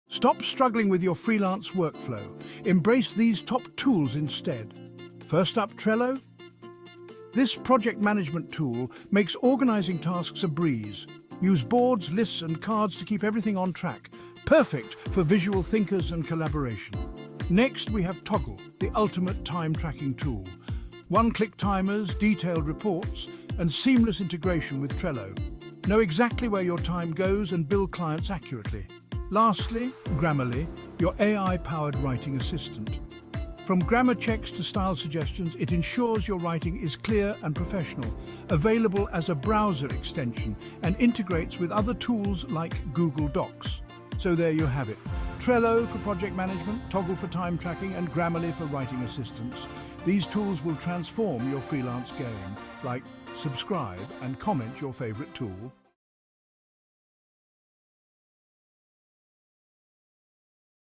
Top 3 Tools to Boost Your Freelance Productivity
Hey, freelancers! Are you ready to take your productivity to the next level? In this video, I’m sharing my top 3 must-have tools that will help you stay organized, efficient, and on top of your game. Whether you're a seasoned pro or just starting your freelance journey, these tools will make a HUGE difference in your workflow.

💬 Let’s Connect!
Have a favorite productivity tool? Share it in the comments below! I’d love to hear what works for you. 👇